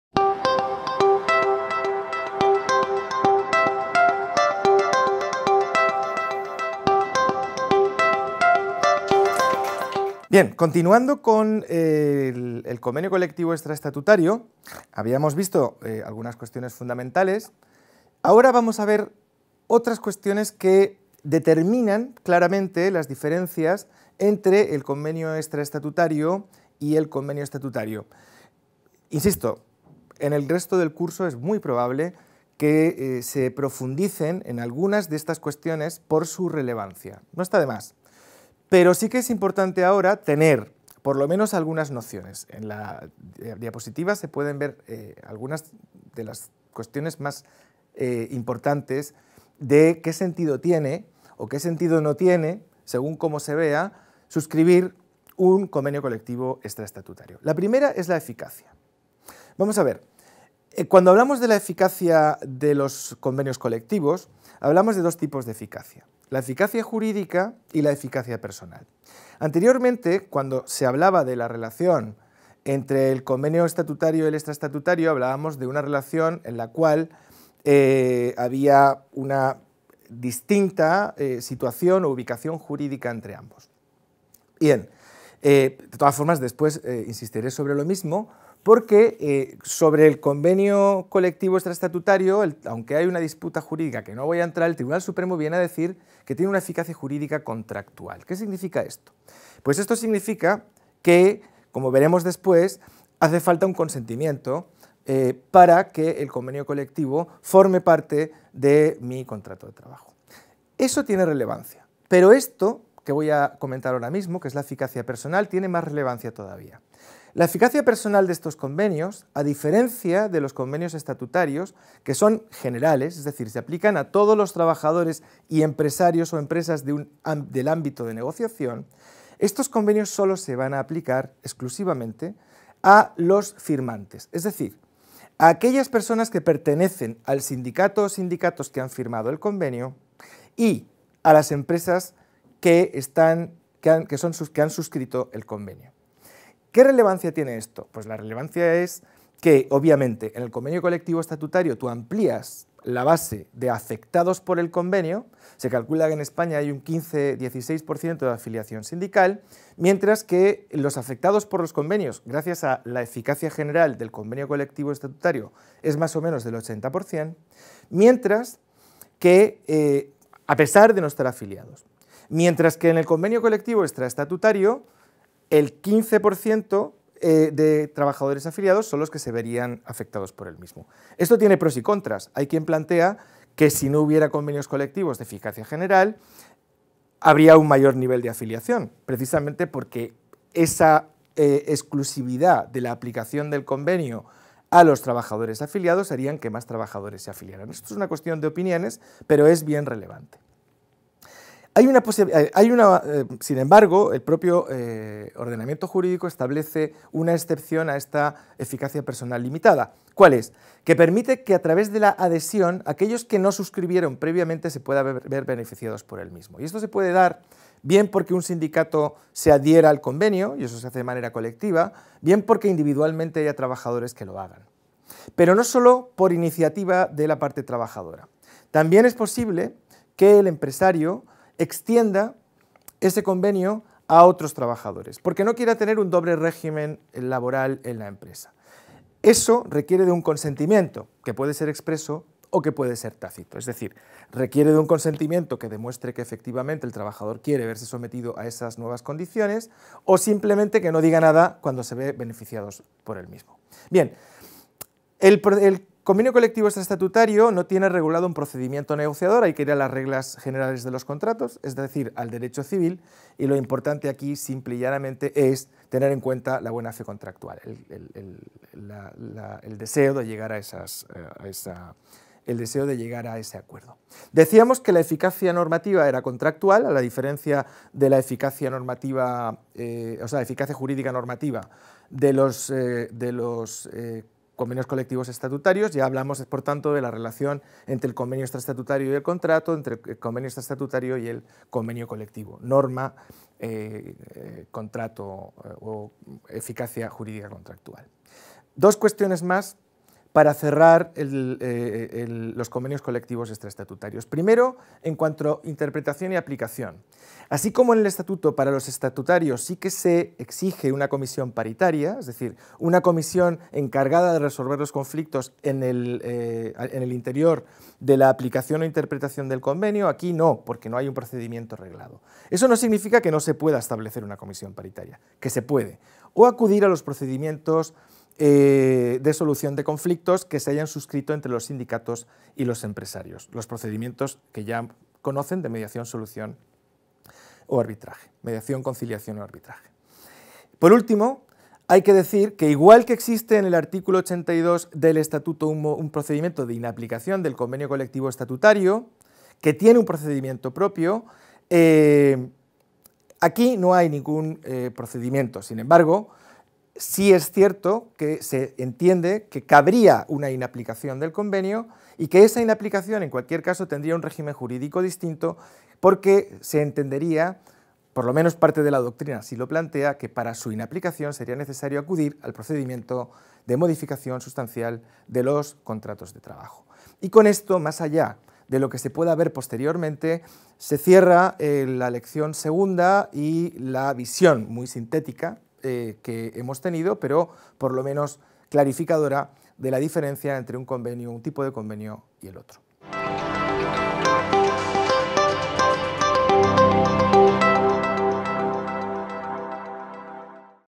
2.7 El convenio colectivo extraestatutario II
Vídeo de la 7ª lección del Módulo 2 : "Acuerdos colectivos" del MOOC: "La negociación colectiva".